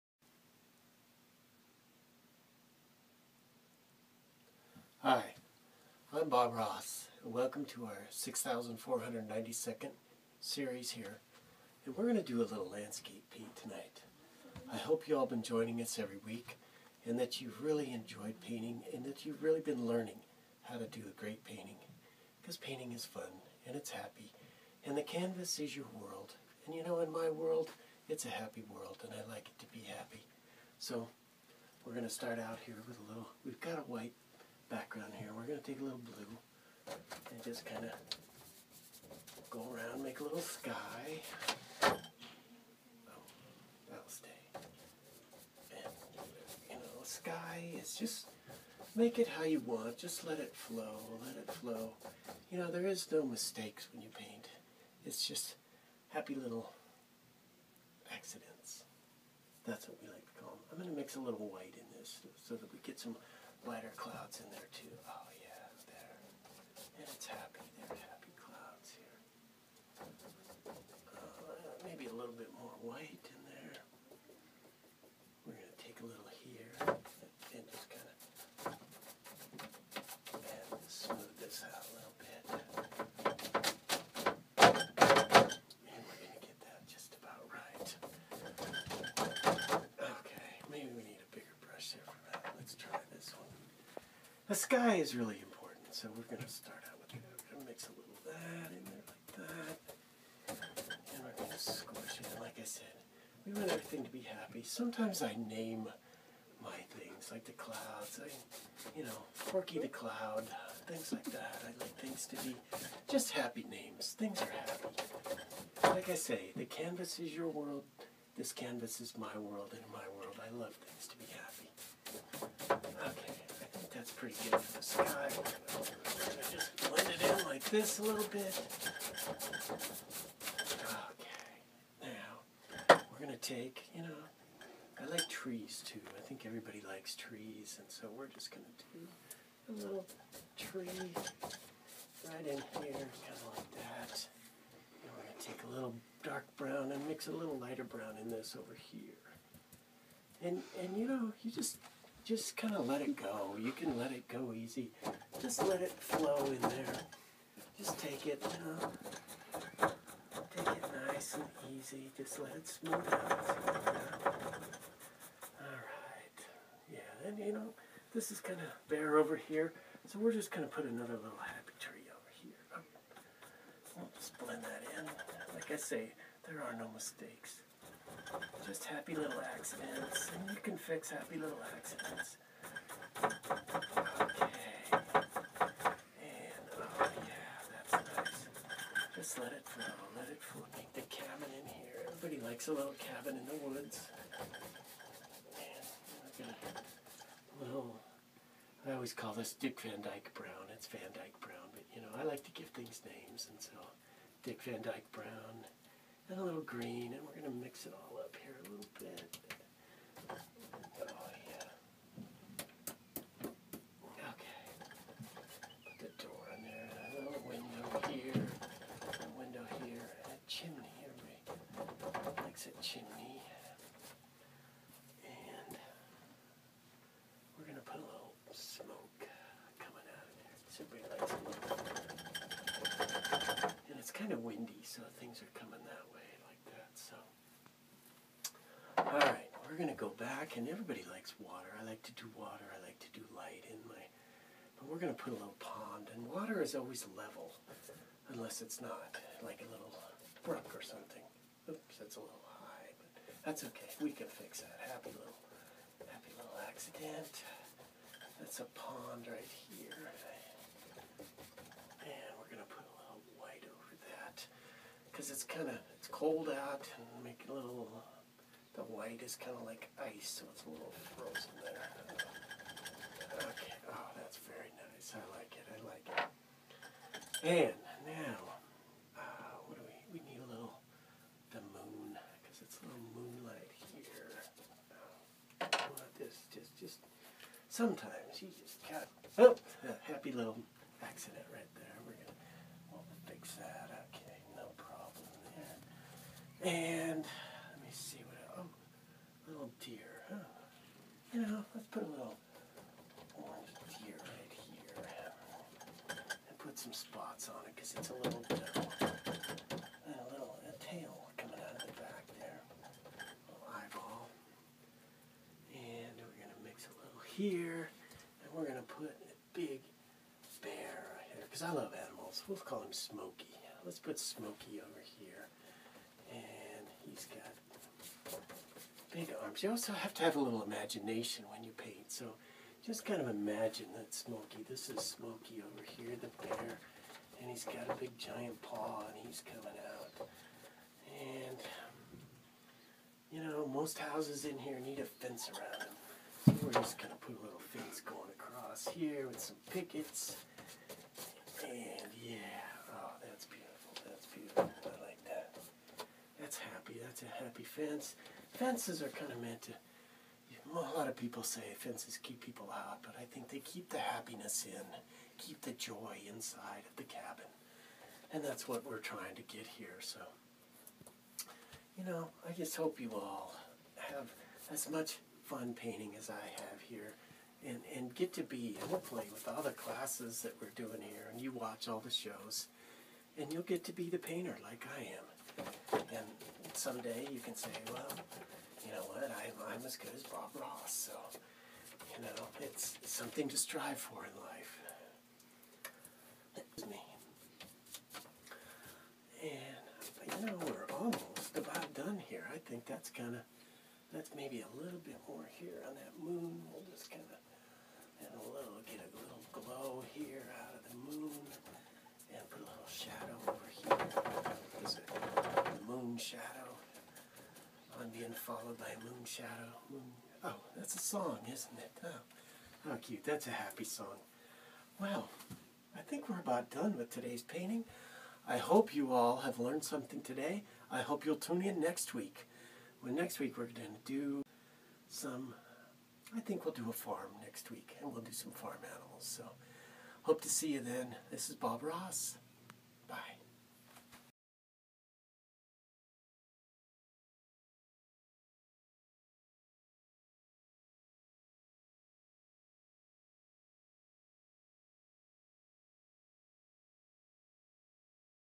The Joy of Christmas Painting
SouthGate Church's Christmas Production 2013.  Video of Bob Ross Parody.